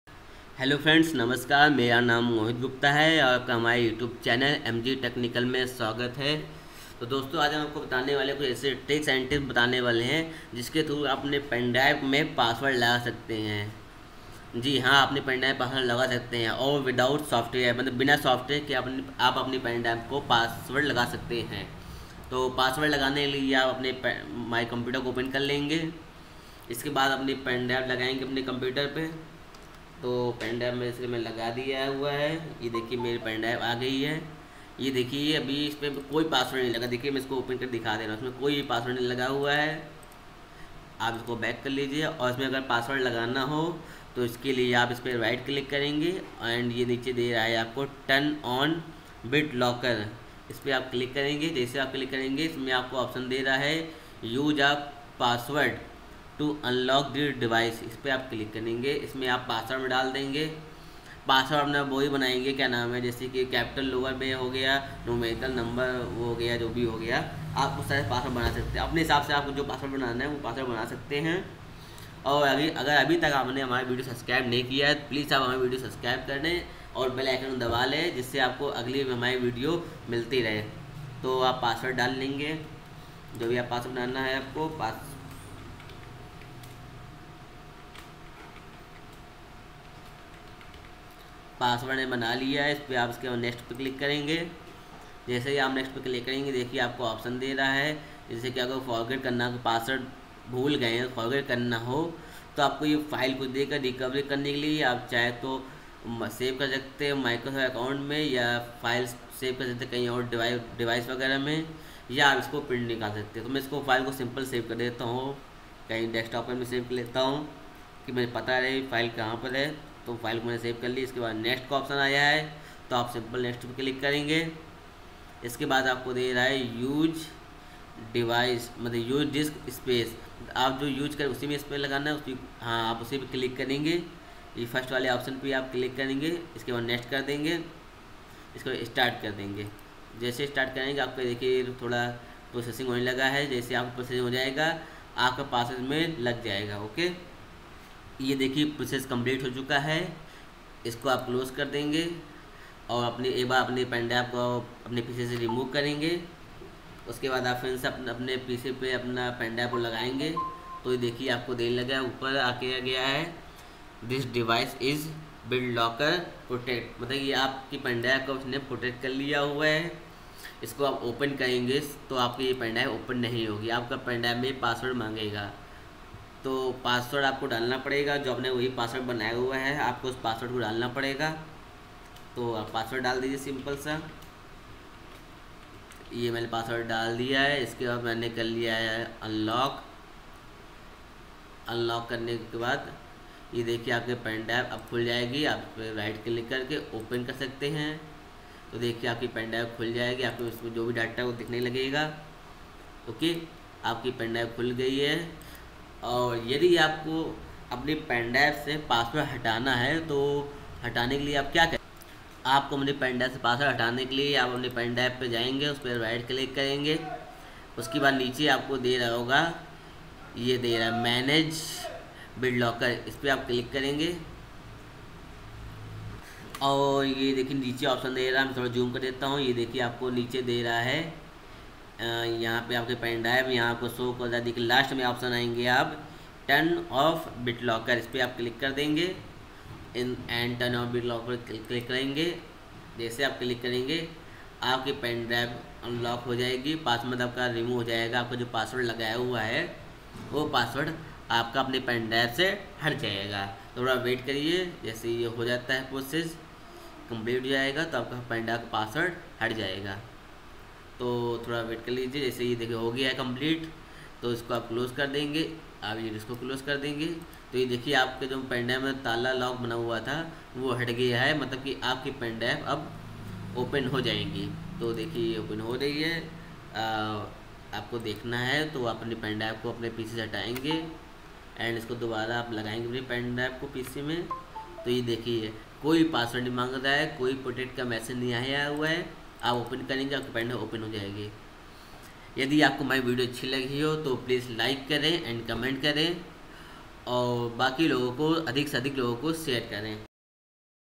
How to set password in pendrive without any software
Hello friends aaj ki video maine apko bataya hai ki aap kis tarah se aap apne pendrive me password laga sakte hai aur apne pendrive ko safe kar sakte hai ye bahut hi simple trick or tips hai to ager aap bhi jaanna changte hai to iss video ko last tak jarur dekhe .

Thanks Regards
Mohit Gupta